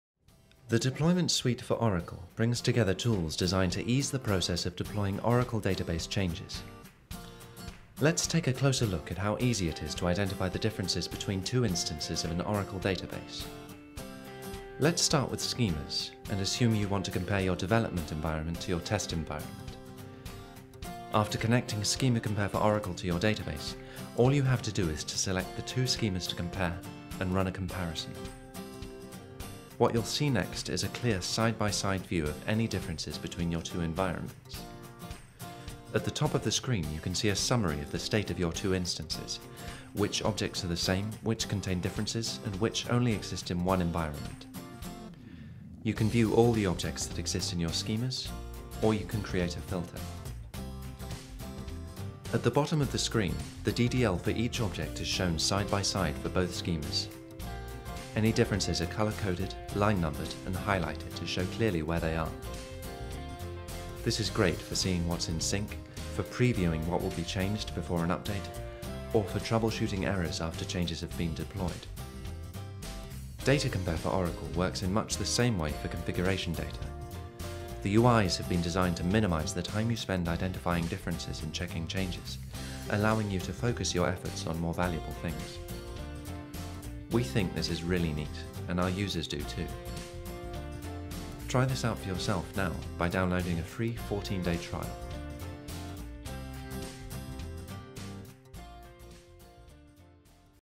Comparing Two Oracle Schema | Redgate
The Deployment Suite for Oracle brings together tools to ease the process of deploying Oracle database changes.

After connecting Schema compare for Oracle to your database, all you do is select the two schemas to compare and run a comparison.

You’ll then be able to view a clear side-by-side view of any differences between your two environments.